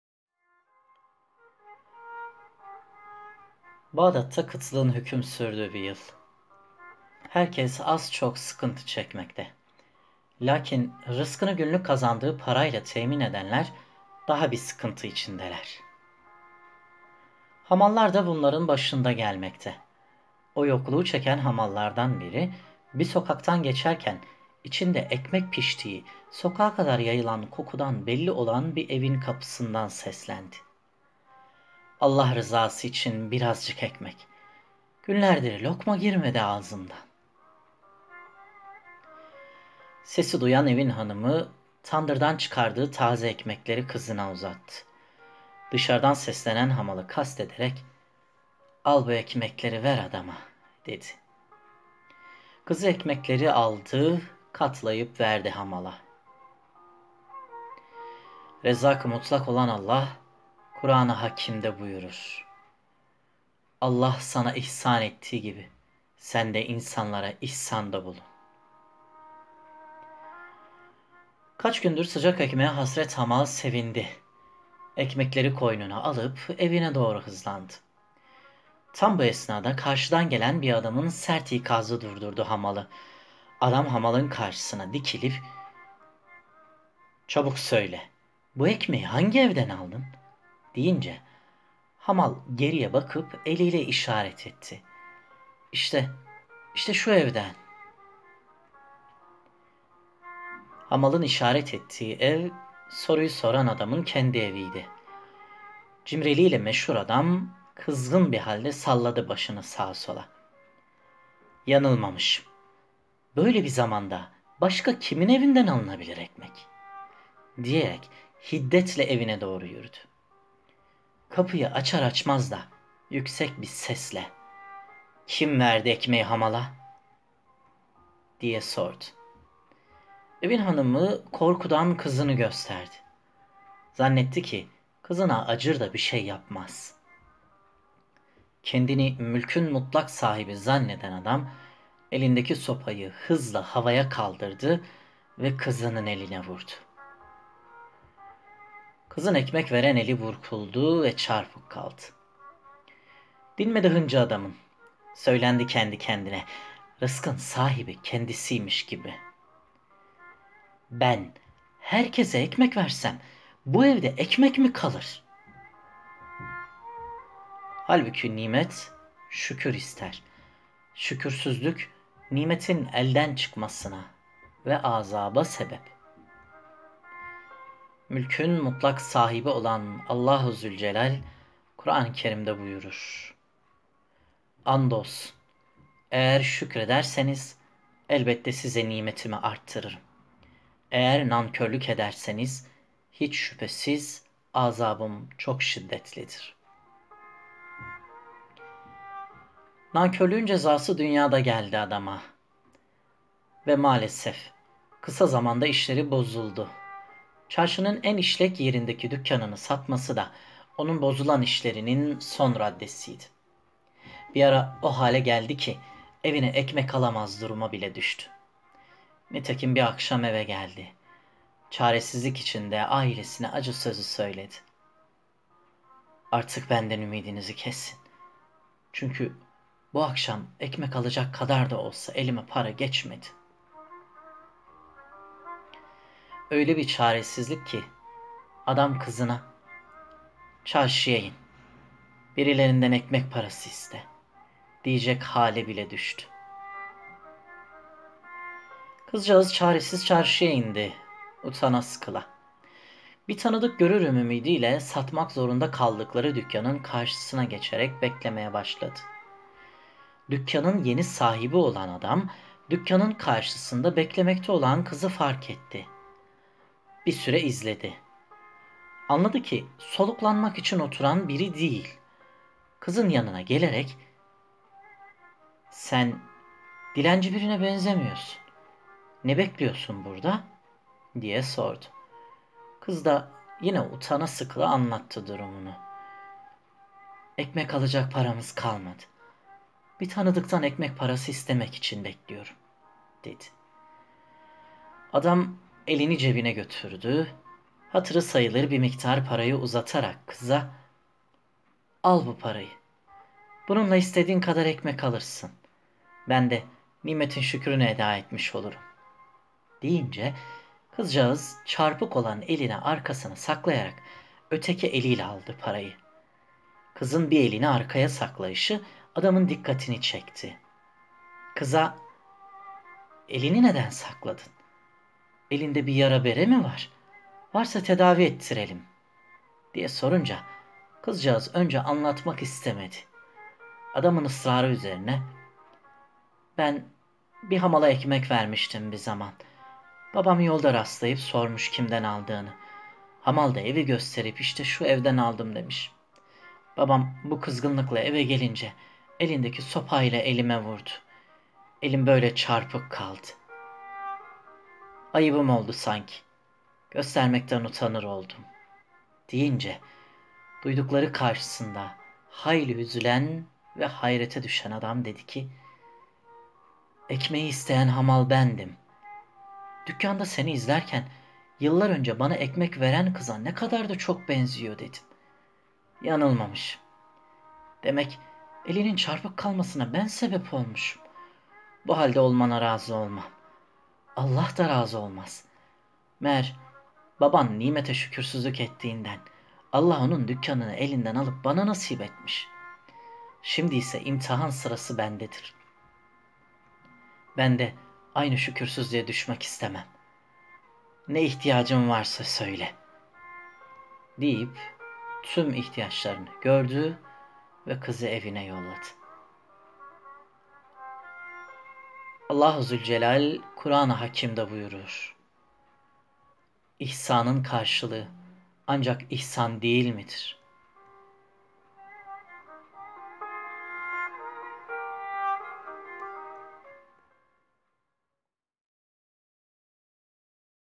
Cömertlik I Bir Dilim Ekmek I Hikaye/Kıssa/Menkîbe I Dini Hikayeler
Menkıbe
Bağdat’da kıtlığın hüküm sürdüğü bir Yıl. Herkes az çok sıkıntı çekmekte, lakin rızkını günlük kazandığı parayla temin edenler daha bi sıkıntı içindeler. Hamallarda bunların başında gelmekte... O yokluğu çeken  Hamallardan biri, bi sokaktan geçerken İçinde ekmek piştiği, sokağa kadar yayılan kokudan belli olan bir evin kapısından seslendi,…
– Allah rızası için birazcık ekmek. Günlerdir lokma girmedi ağzımdan.
Sesi duyan Evin hanımı, tandırdan çıkardığı taze ekmekleri  kızına uzattı.
Dışarıdan seslenen hamalı kastederek…
-“Al bu ekmekleri ver  adama” dedi.
 Kız ekmekleri aldı,  katlayıp verdi  hamala…
Rezzakı Mutlak olan Allah Kur’anı Hakimde Buyurur.
“Allah sana ihsan ettiği gibi, sen de insanlara ihsanda bulun”
Kaç gündür sıcak ekmeğe hasret Hamal sevindi. Ekmekleri koynuna alıp Evine doğru hızlandı. Tam bu esnada karşıdan gelen bir adamın sert ikazı durdurdu hamalı:
Adam hamalın karşısına dikilip,
– Çabuk söyle, bu ekmeği hangi evden aldın? Diye sorunca
Hamal Geriye bakıp eliyle işaret etti:
– İşte,işte şu evden. Dedi adama
Hamalın işaret ettiği ev soruyu soran Adamın kendi eviydi.
Cimriliğiyle meşhur Adam, kızgın bi halde  salladı başını sağa sola:
– Yanılmamışım, böyle bir zamanda başka kimin evinden alınabilir ekmek?  diyerek Hiddetle evine doğru yürüdü.
Kapıyı açar açmaz da  sordu:
– Kim verdi ekmeği hamala?
Evin Hanımı, korkudan kızını gösterdi. Zannetti ki kızına acır da, bir şey yapmaz.
Kendini Mülkün Mutlak sahibi zanneden Adam, Elindeki sopayı hızla havaya kaldırdı ve  kızının eline vurdu, kızın ekmek veren eli burkuldu ve çarpık kaldı.
Dinmedi hıncı Adamın Söylendi kendi kendine.
Rızkın sahibi kendisiymiş gibi,
– Ben, herkese ekmek versem, bu evde ekmek mi  kalır?
Halbuki nimet şükür ister. Şükürsüzlük, nimetin elden çıkmasına ve azaba sebeptir.
Mülkün Mutlak Sahibi olan  ALLahu Zülcelall  Kuran-ı Kerimde buyurur.
“Andolsun, eğer şükrederseniz elbette size nimetimi artırırım. Eğer nankörlük ederseniz, hiç şüphesiz azabım çok şiddetlidir.”" İbrahim .7-8